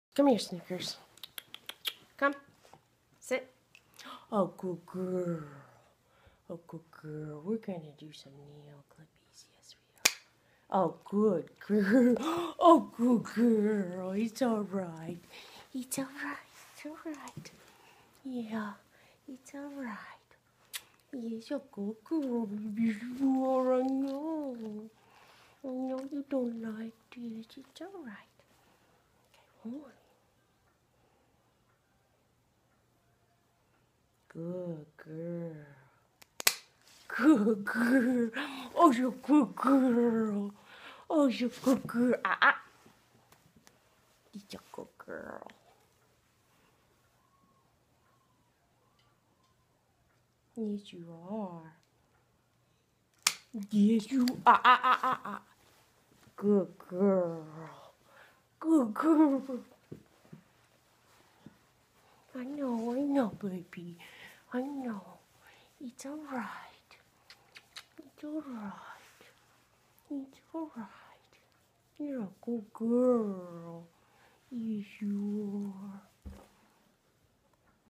Snickers the border collie nail trim
My border collie doesn't understand why I am clipping her nails